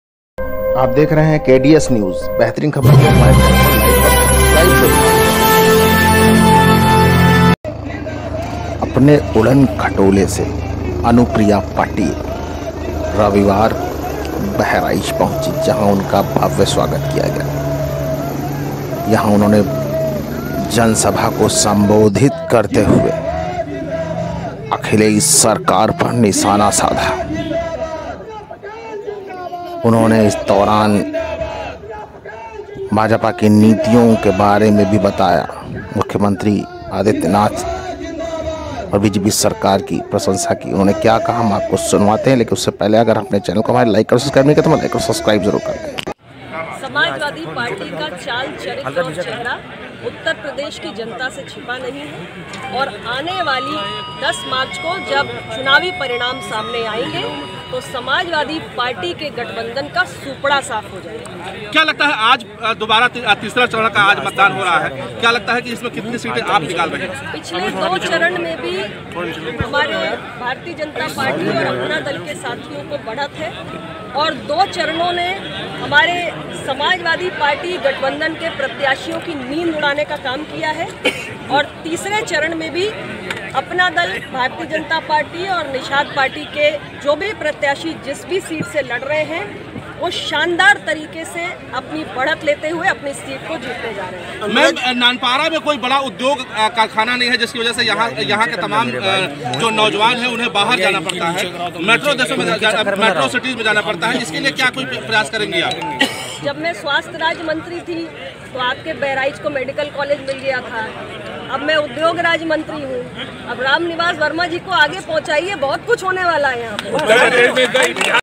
10 मार्च को समाजवादी पार्टी के गठबंधन का सूपड़ा साफ हो जाएगा- अनुप्रिया पटेल
बहराइच, यूपी

स्लग - समाजवादी पार्टी का चाल चरित्र और चेहरा उत्तर प्रदेश की जनता से छुपा नही है 10 मार्च को समाजवादी पार्टी के गठबंधन का सूपड़ा साफ हो जाएगा- अनुप्रिया पटेल

एंकर - अपना दल भाजपा और निषाद पार्टी के गठबंधन के बहराइच की नानपारा विधानसभा से प्रत्याशी रामनिवास वर्मा के समर्थन में जनसभा करने नानपारा पहुँची अपना दल की राष्ट्रीय अध्यक्ष और केंद्रीय वाणिज्य एवं उद्योग राज्यमंत्री अनुप्रिया पटेल विपक्षी पार्टियों पर जमकर हमलावर हुई/अनुप्रिया पटेल ने कहा कि समाजवादी पार्टी का चाल चरित्र और चेहरा उत्तर प्रदेश की जनता से छुपा नही है आने वाली 10 मार्च को जब चुनावी परिणाम सामने आएंगे तब समाजवादी पार्टी के गठबंधन का सूपड़ा साफ हो जाएगा/ उन्होंने कहा कि पिछले 2 चरण में भारतीय जनता पार्टी और अपना दल के साथियों को बढ़त मिल रही है/ 2 चरणों ने समाजवादी पार्टी गठबंधन के प्रत्याशियों की नींद उड़ाने का काम किया है और तीसरे चरण में भी भारतीय जनता पार्टी ,अपना दल और निषाद पार्टी के जो भी प्रत्याशी जिस भी सीट से चुनाव लड़ रहे हैं वो शानदार तरीके से बढ़त लेते हुए अपनी सीट को जीतेंगे/ उन्होंने कहा कि जब मैं स्वास्थ्य राज्यमंत्री थी तो बहराइच को मेडिकल कालेज मिला था अब मैं उद्योग राज्य मंत्री तो भी बहराइच को बहुत कुछ मिलेगा /उन्होंने जनसभा में मौजूद हजारों लोगों से अपना दल  प्रत्याशी रामनिवास वर्मा को भारी मतों से जिताने की अपील की/

बाईट - अनुप्रिया पटेल ( केंद्रीय वाणिज्य एवं उद्योग राज्यमंत्री)